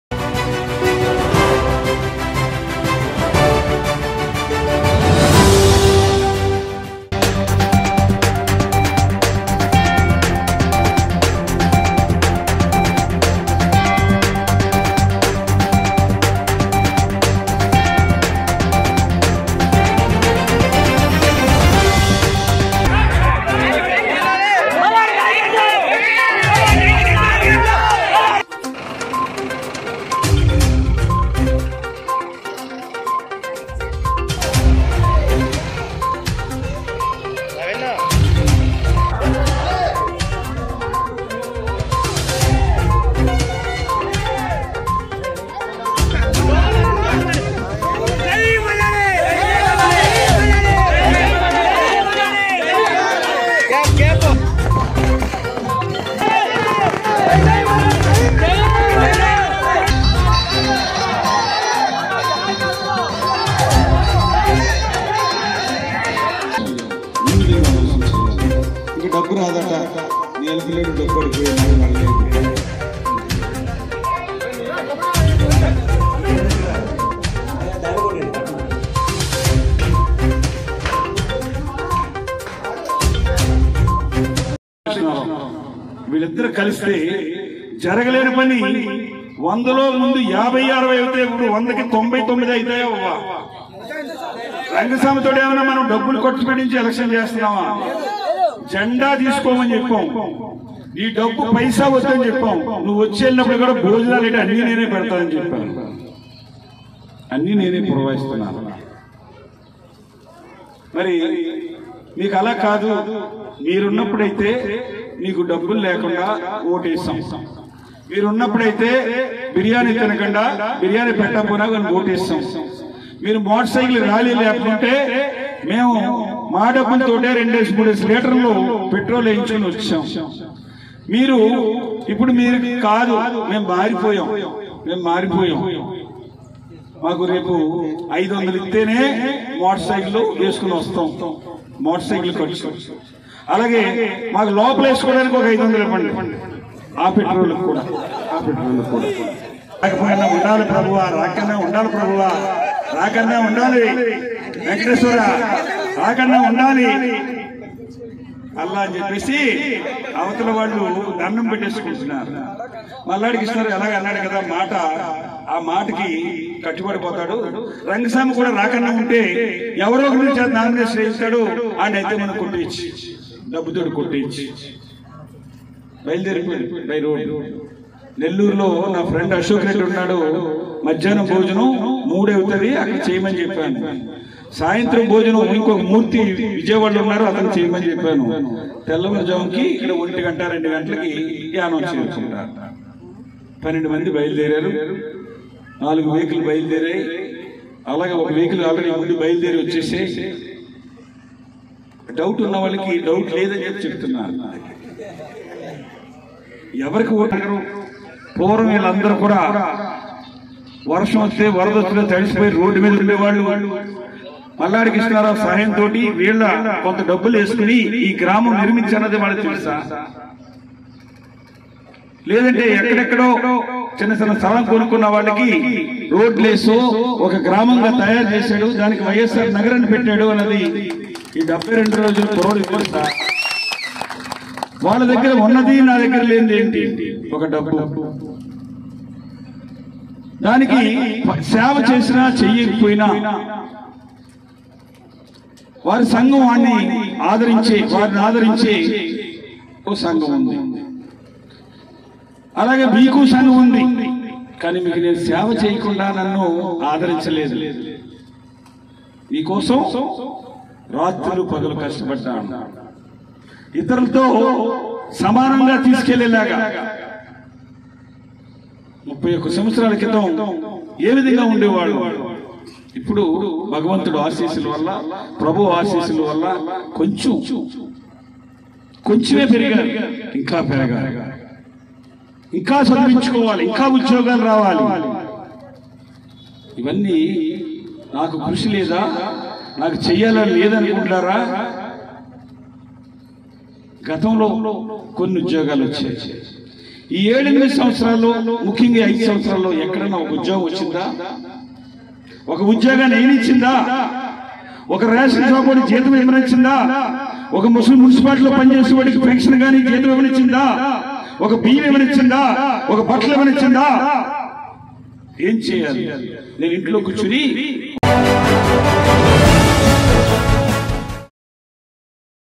NN1 NEWS:తూర్పుగోదావరి జిల్లా,ముమ్మిడివరం.యానం అసెంబ్లీ ఎన్నికల్లో రంగస్వామి కి మద్దతుగా మాజీ మంత్రి
తూర్పుగోదావరి జిల్లా,ముమ్మిడివరం.యానం అసెంబ్లీ ఎన్నికల్లో రంగస్వామి కి మద్దతుగా మాజీ మంత్రి మల్లాడి కృష్ణారావు గ్రామాల్లో ఎన్నికల ప్రచారం.